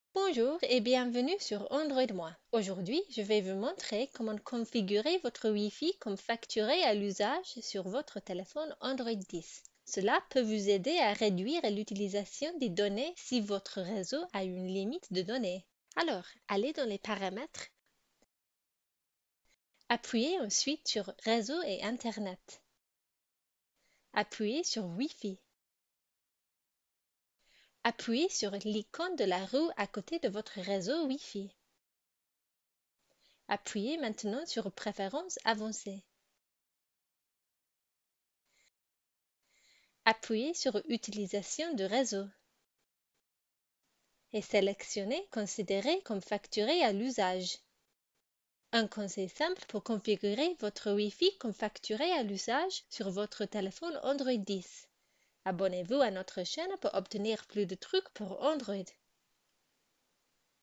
Comment configurer votre WiFi comme facturé a l'usage sur Android 10
Cela peut vous aider à réduire l'utilisation des données si votre réseau a une limite de données.
Pour ce faire, accédez aux paramètres
Appuyez ensuite sur "Réseau et Internet"
Appuyez sur Wi-Fi
Appuyez sur l'icône de la roue à côté de votre réseau wifi
Appuyez maintenant sur préférences Avancé"
Appuyez sur "Utilisation du réseau"
Et sélectionnez "Considérer comme facturé a l'usage"